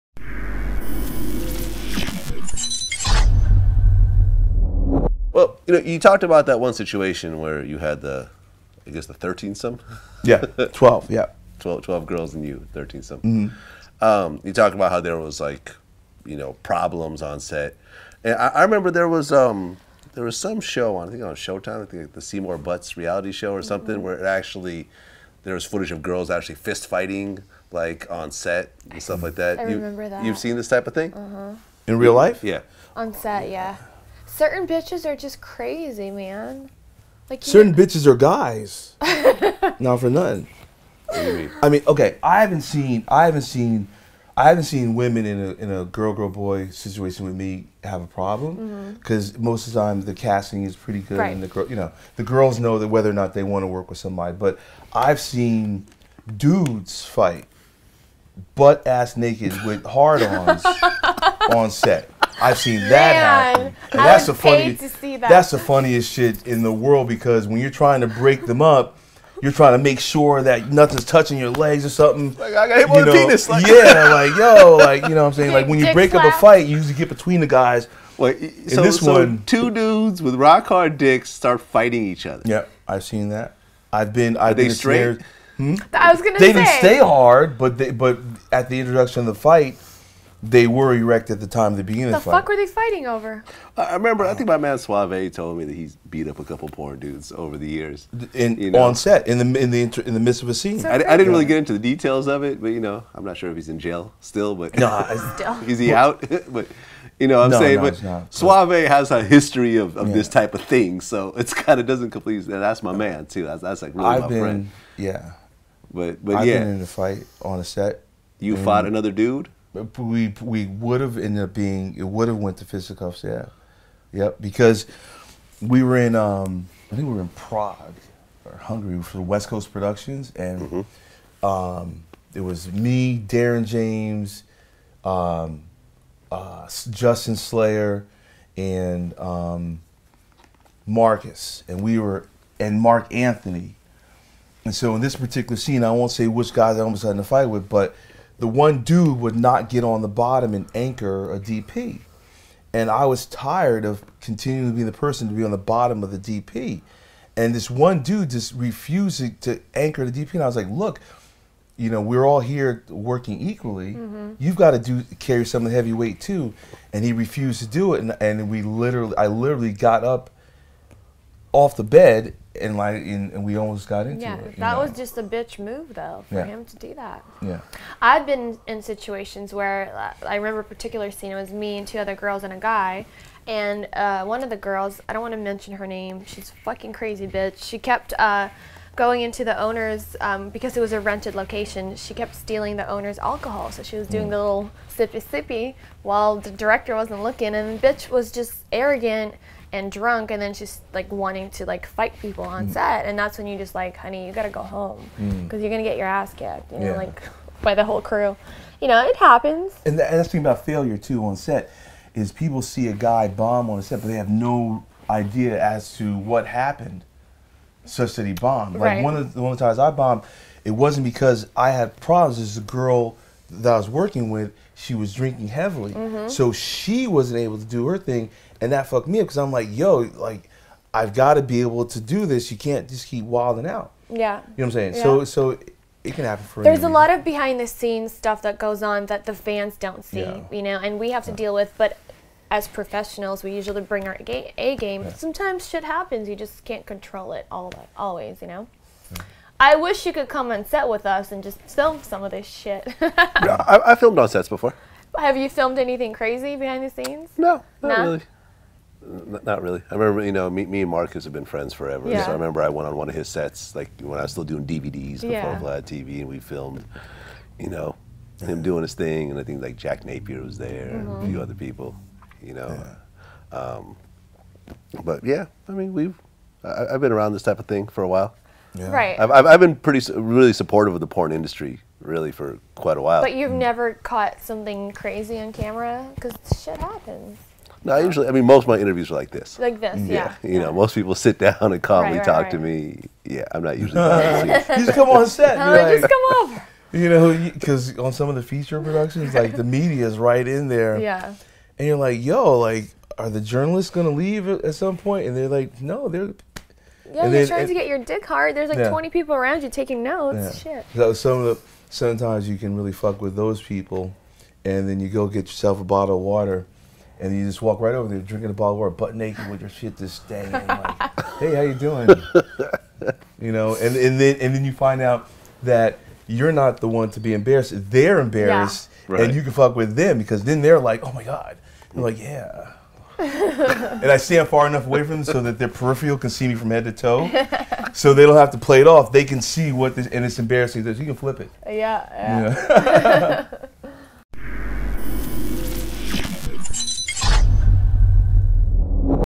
Flashback: Nikki Benz and Lex Steele Recall Multiple Fights While Filming
In 2015, Nikki Benz and Lex Steele spoke on the fights that they've witnessed while filming. "I've seen dudes fight butt-a** naked on set," Lex recalled. "That's the funniest s*** in the world because when you 're trying to break them up, you're trying to make sure that nothing's touching your leg or something."

Check out his crazy story as well as one from Nikki Benz above.